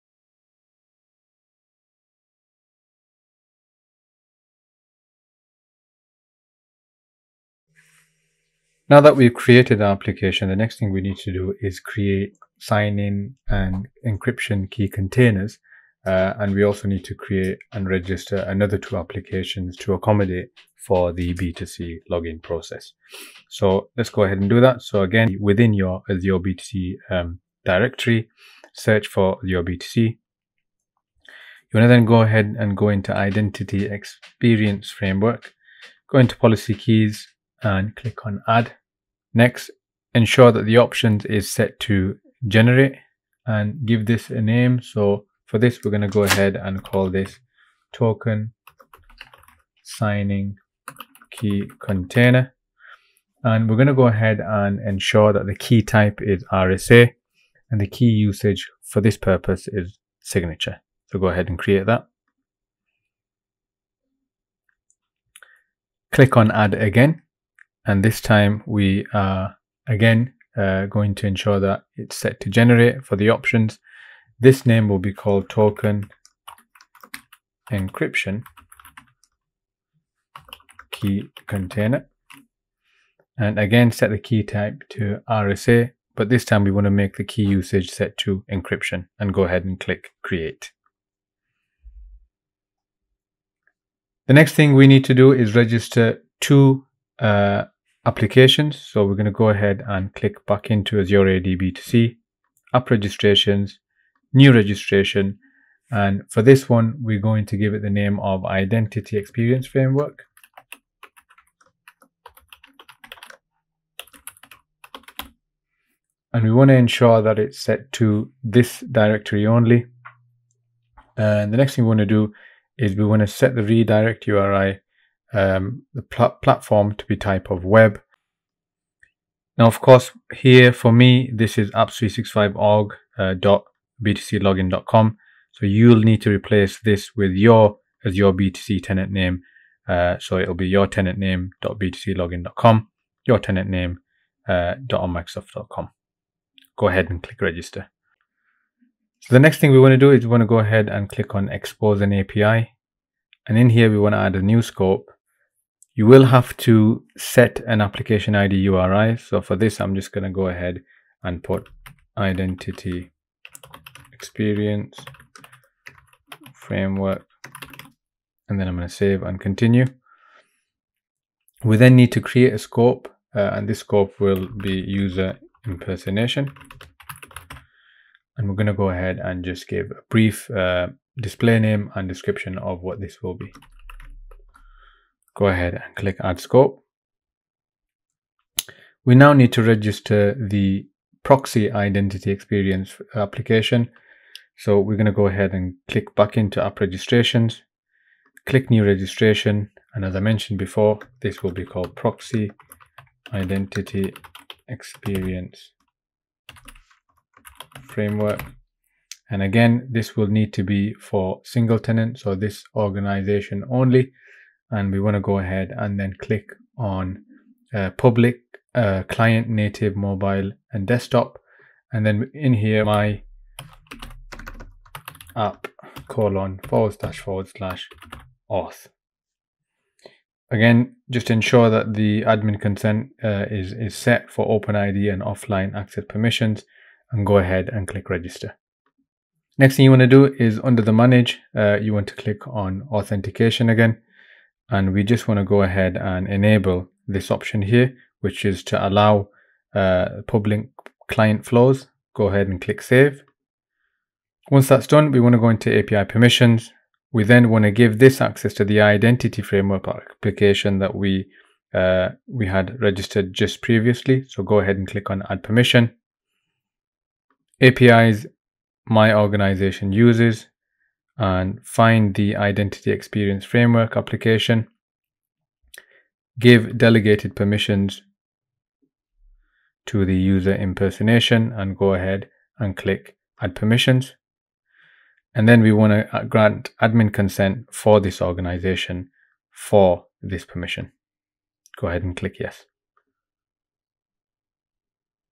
Creating Azure AD B2C Custom Policies - Part 2 - Setting up Identity Experience Framework
This is part 2 of a series of videos detailing steps on how to create an azure B2C custom policy.

In this video we setup the Identity Experience Framework